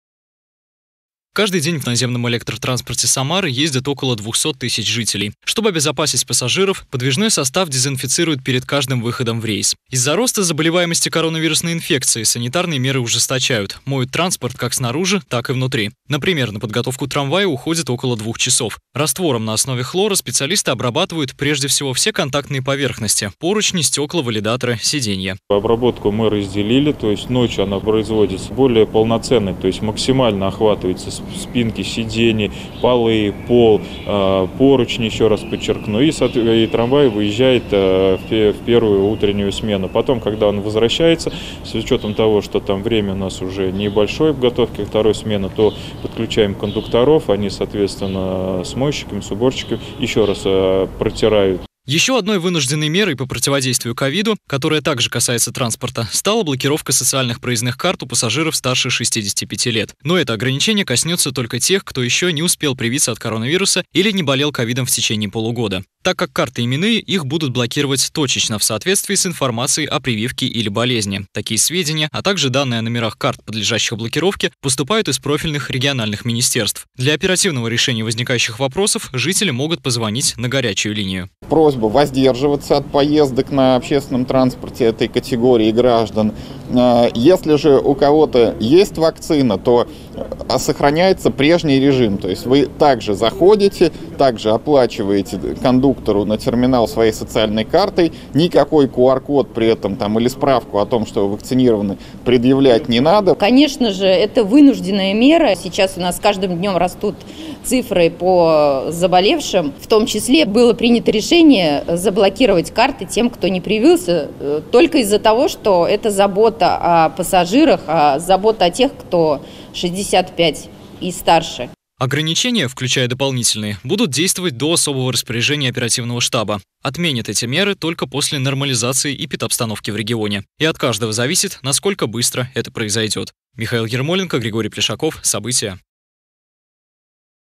В Самаре усилили работу по дезинфекции общественного транспорта
В Самаре усилили работу по дезинфекции общественного транспорта. Автобусы, трамваи и троллейбусы моют чаще и чище. Санитарные нормы ужесточают, чтобы в регионе не пришлось вводить локдаун. Одна из новых мер, которую ранее приняли на заседании штаба по борьбе с коронавирусной инфекцией, - это блокировка карт для пенсионеров. Жители старше 65 лет, не переболевшие ковидом и не сделавшие прививку, не смогут оплатить проезд транспортной картой. Подробнее о нововведениях расскажем в сюжете.